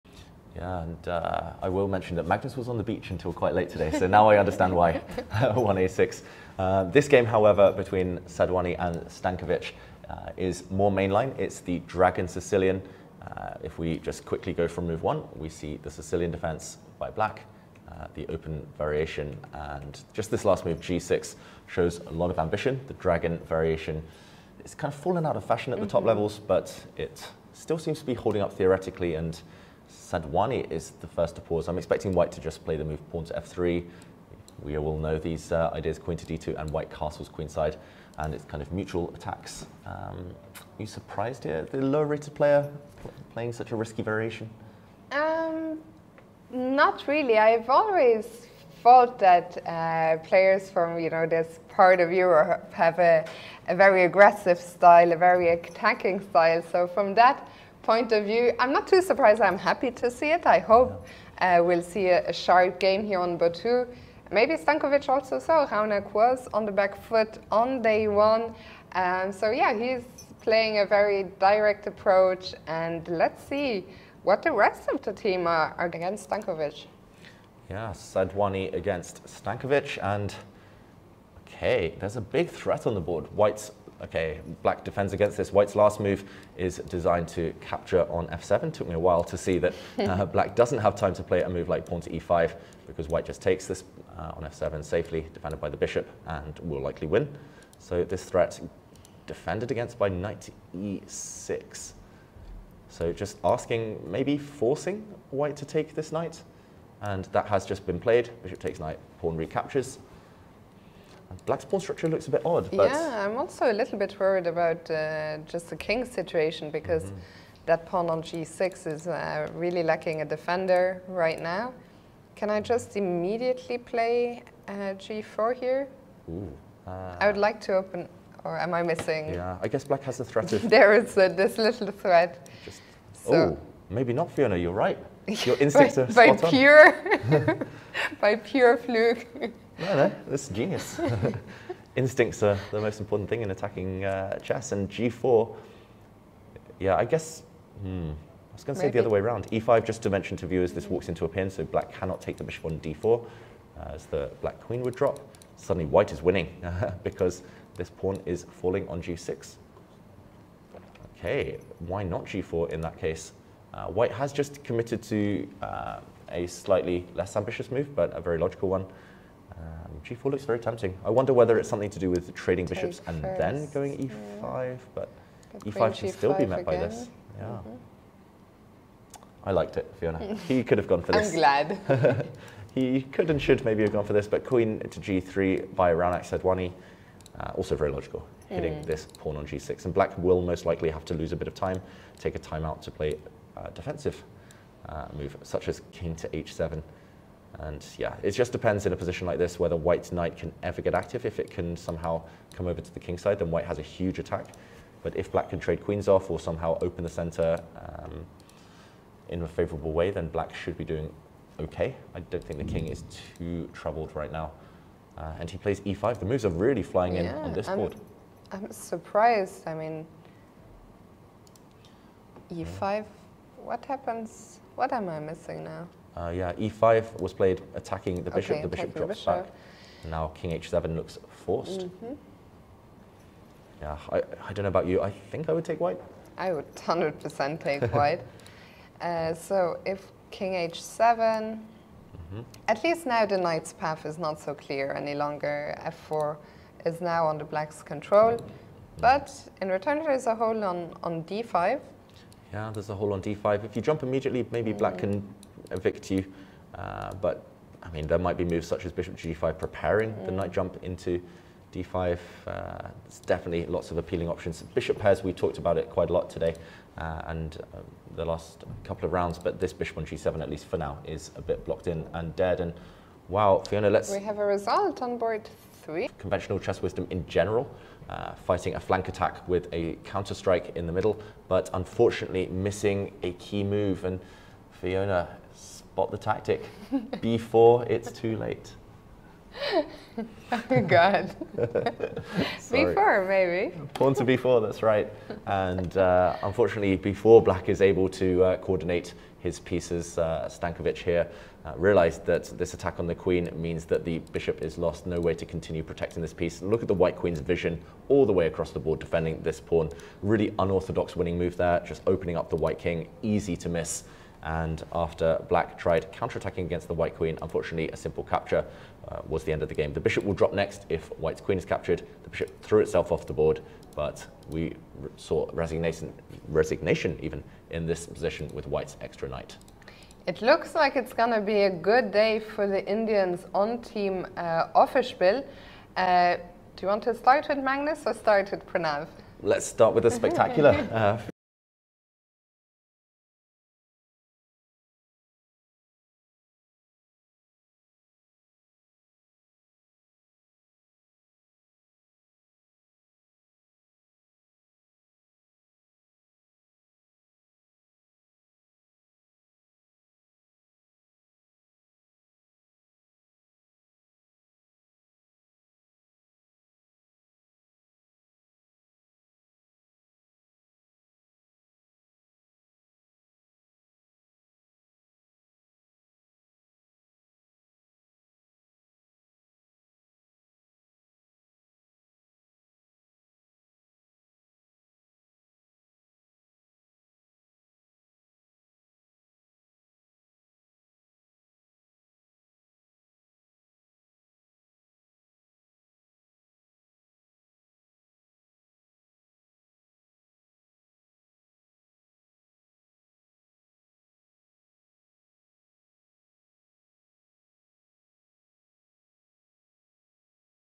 "Dragon Slayer" Sadhwani, Raunak - Stankovic, Milos Euro Cup Chess. !-
Commentary, Interview and Game Replay

Euro Cup Chess RD3